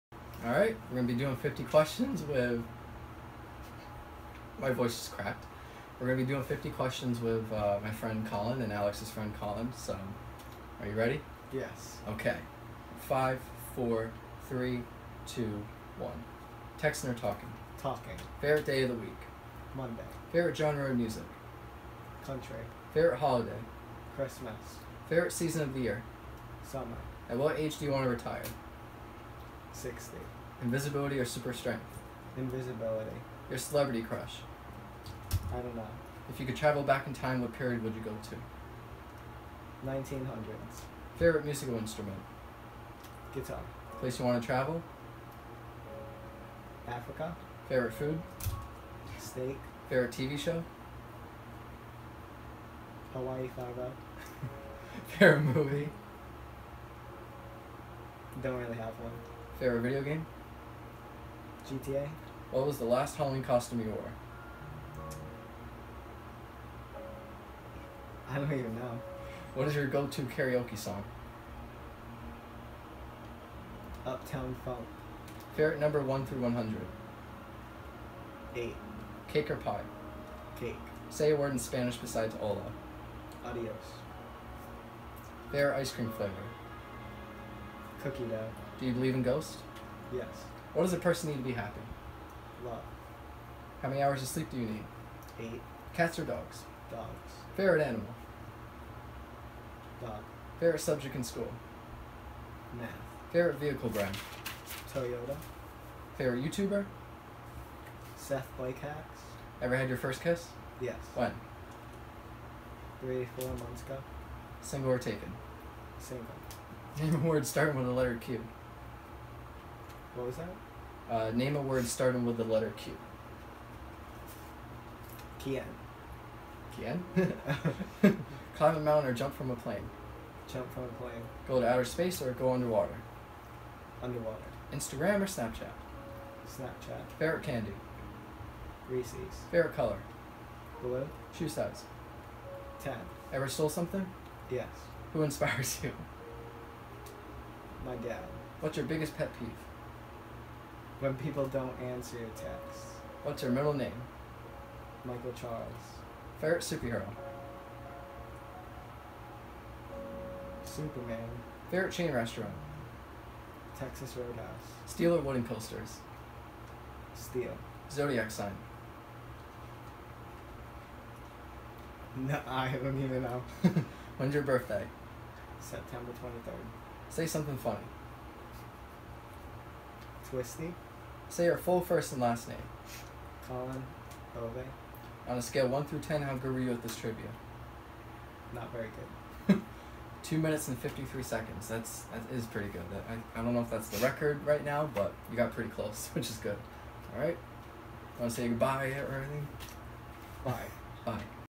SpeedRound Challenge: Collin’s Interview
My 50 question speed-round challenge continues with interviewing my friend from cross country, Collin Boisvert!

Time result: 2 minutes and 53 seconds